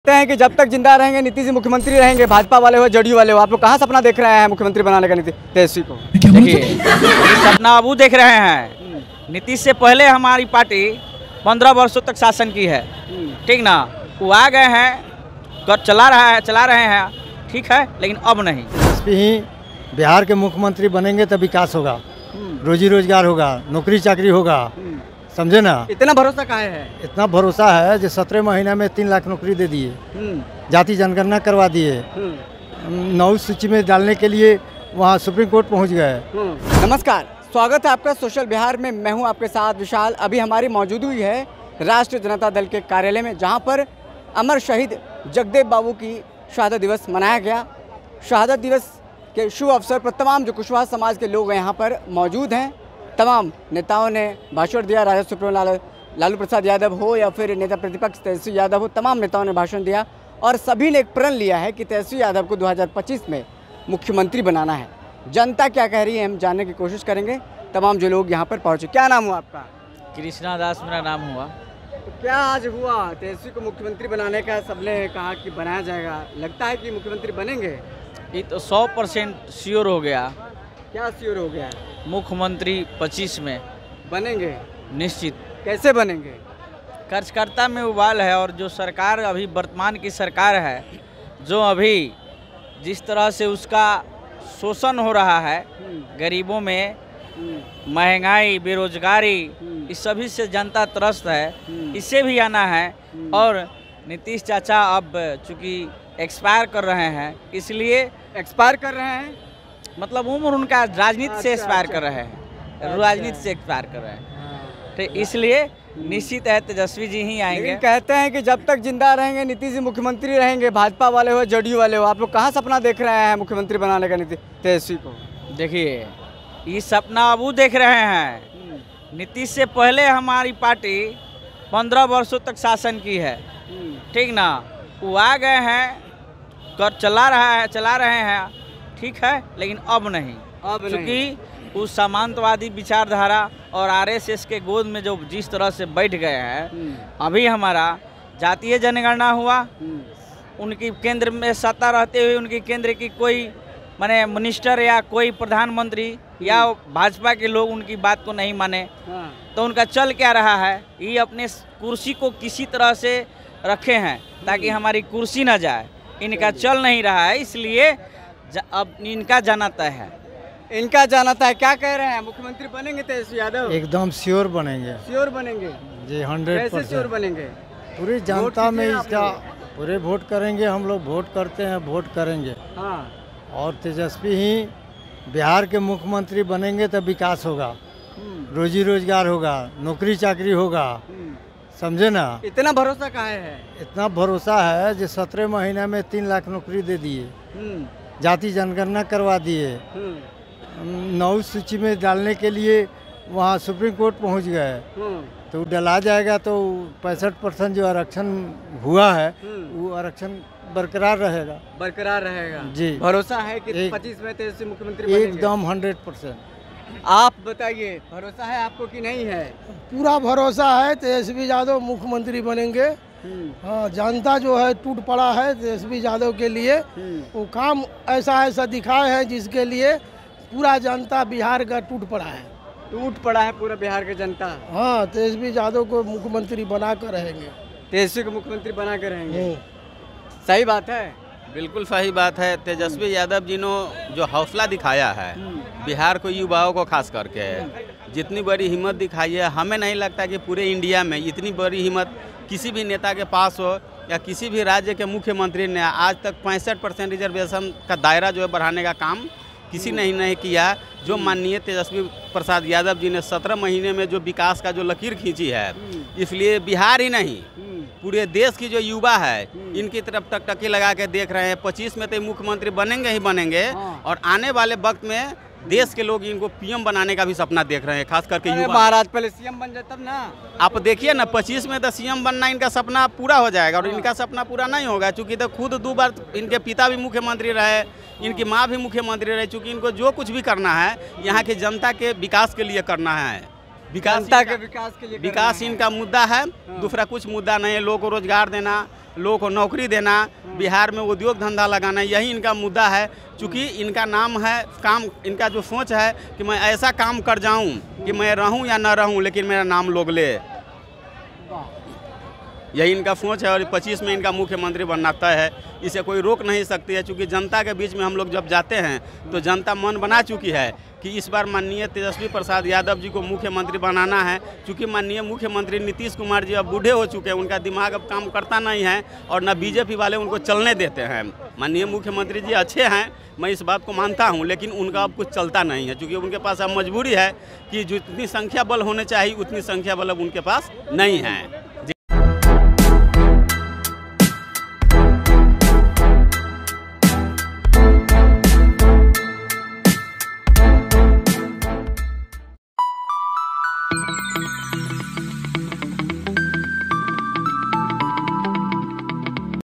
Tejashwi Yadav को CM बनाने कुशवाहा पहुंचे पटना, खूब लगा नारा । देखते रहे Lalu Yadav
RJD Office Tejashwi Yadav
Tejashwi Yadav Speech
Lalu Yadav Hindi Speech
Lalu Yadav loksabha speech
Social Bihar
Social Bihar News